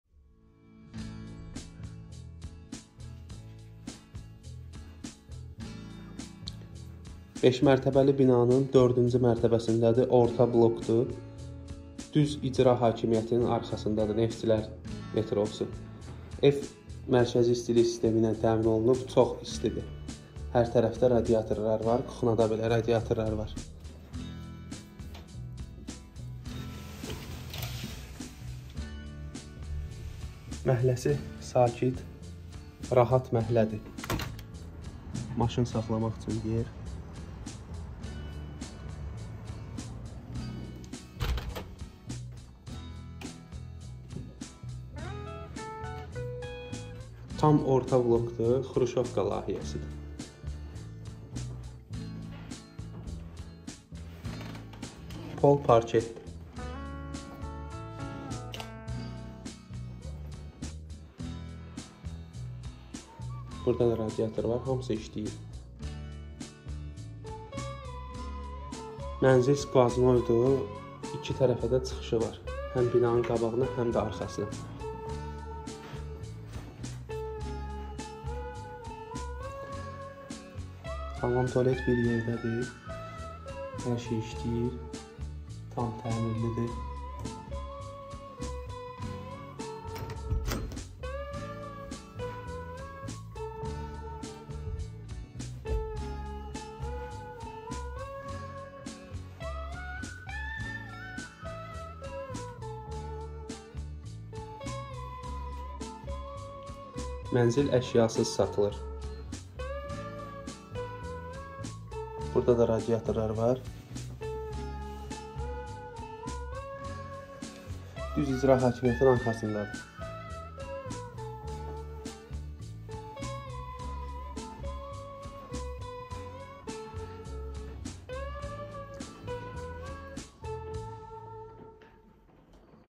Ev Satilir Nefciler 2 Otaq 5/4 Metronun Yani 79 000 Manat Zahid 099-925-99-99
Nizami rayonu Nefciler metrosu yaxinliginda duz Icra hakimiyetinin arxasinda yerlesen 5 mertebeli Das binanin 4 cu mertebesinde qanuni 2 otaqli genis menzil teklif olunur menzil tam temirlidir binanin etrafinda 2 mekteb 2 baxca movcudur metroya cemi 5 deyqelik piyada mesafede yerlesir su qaz isiq daimadir isdilik sistemi Merkezidir sened Kupcadir qeydiyyatda heckim yoxdur alqi satqi olan kimi acarlar tehfil verilecek
Ximdet haqqi 1 %
Qiymet 79 000 manat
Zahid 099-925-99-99